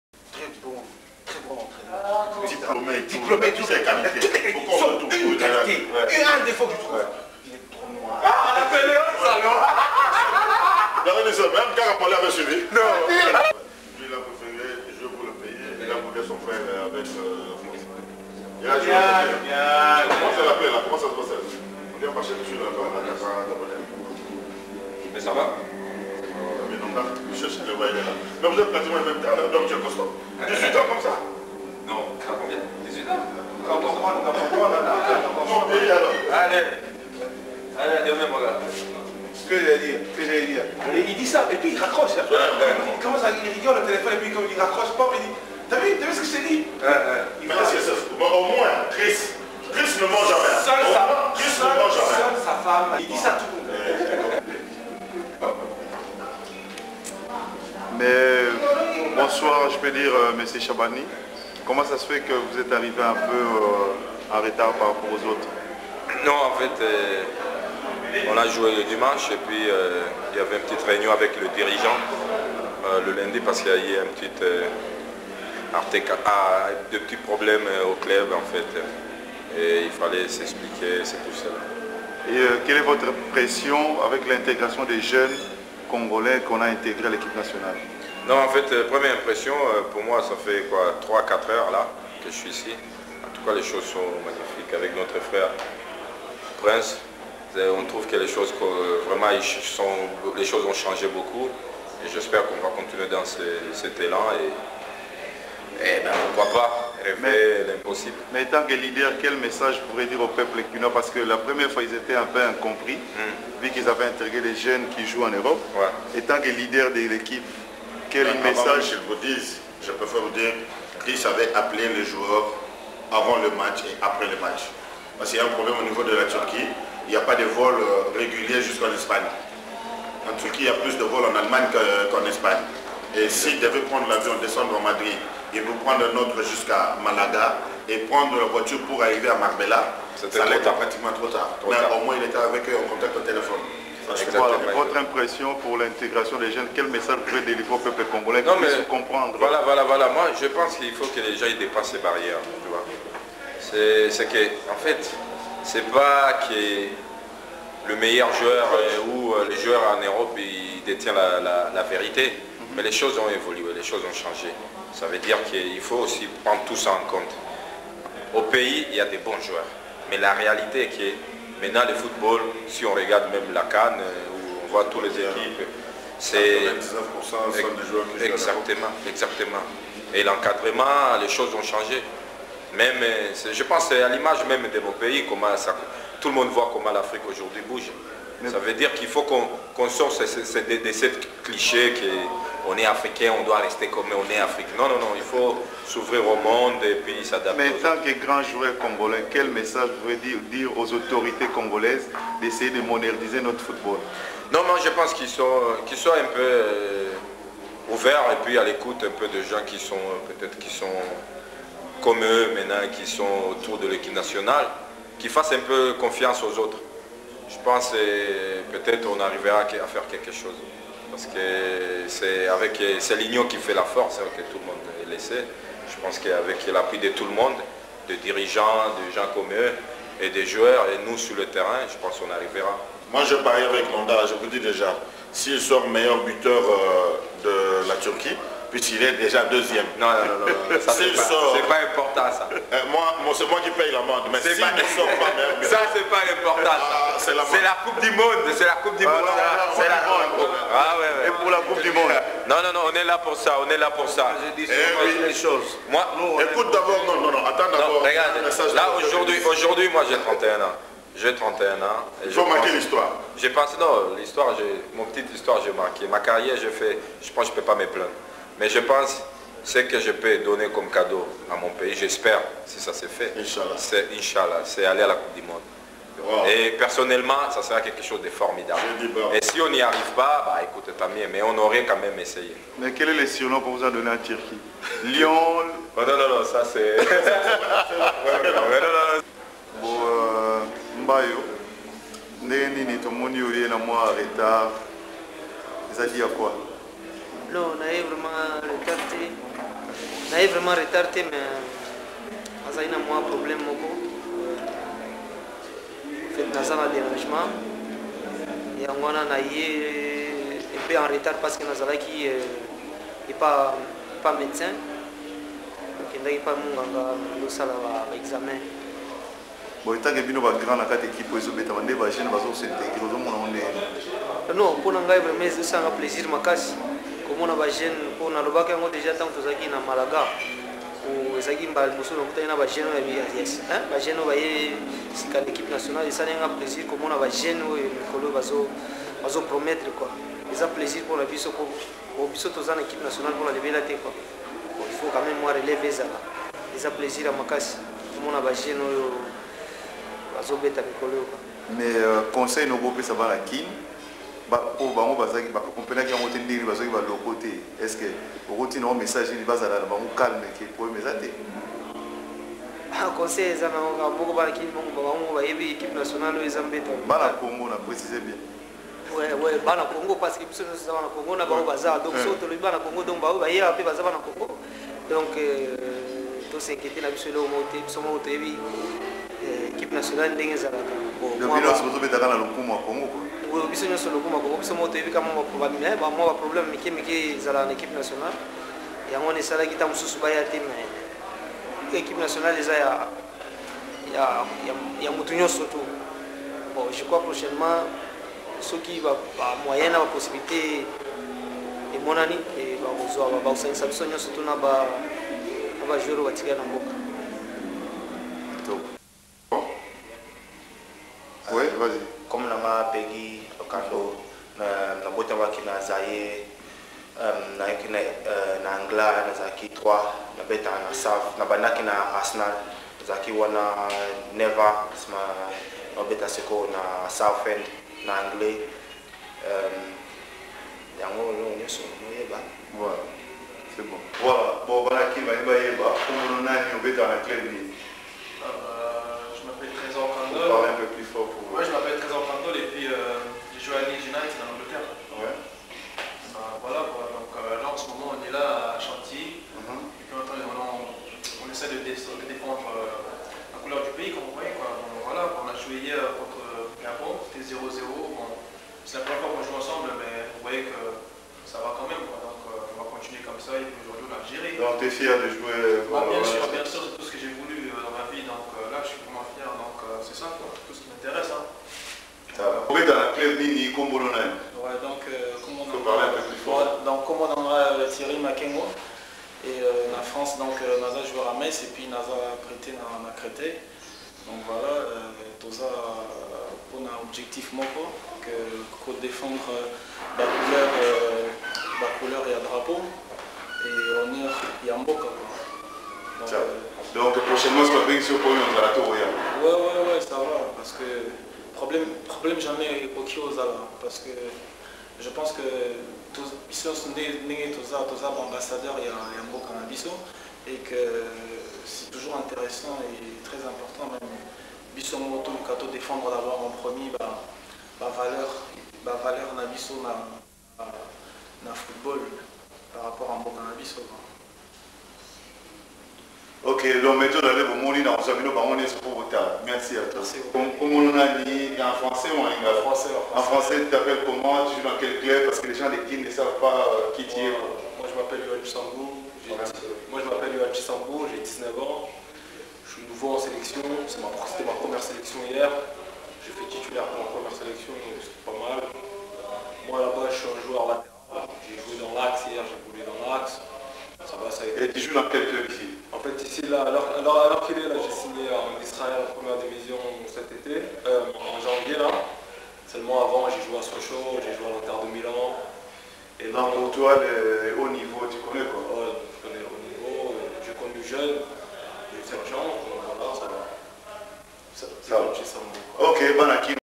INTERVIEW DES LEOPARDS
Par Alain Dizoizo à chantilly avant Algerie - rdc du 26 Mars 2008